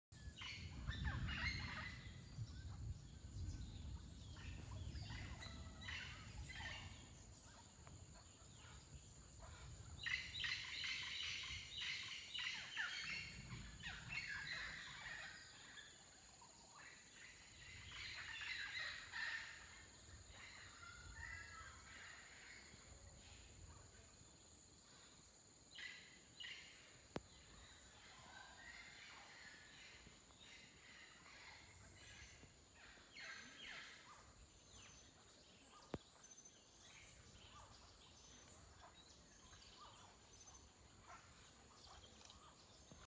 Mundo Perdido, Tikal, Guatemala
Mundo Perdido es una de las zonas más antiguas de Tikal y se cree que en sus inicios, fue un centro de observación astronómica, donde los mayas seguían el movimiento de los astros.